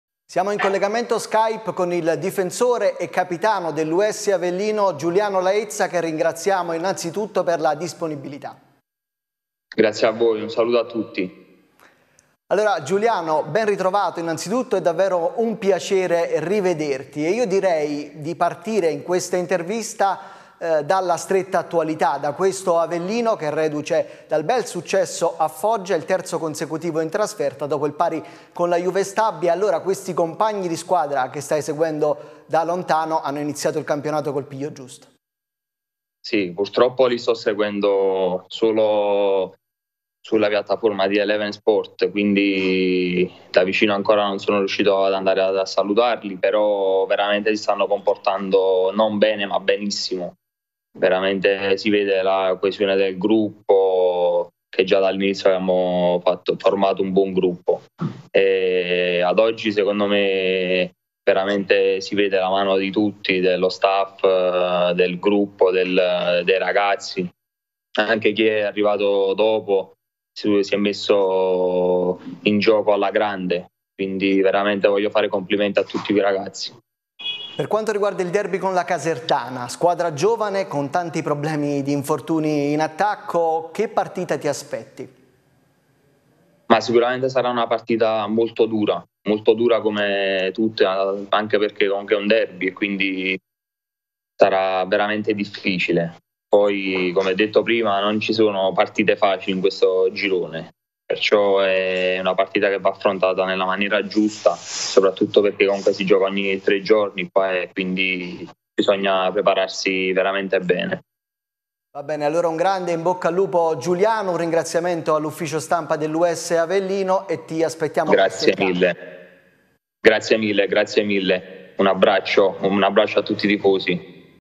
Avellino, Laezza a 696 TV: "Orgoglioso dei miei compagni"
Il capitano, infortunato, ha parlato anche del derby con la Casertana: l'estratto dell'intervista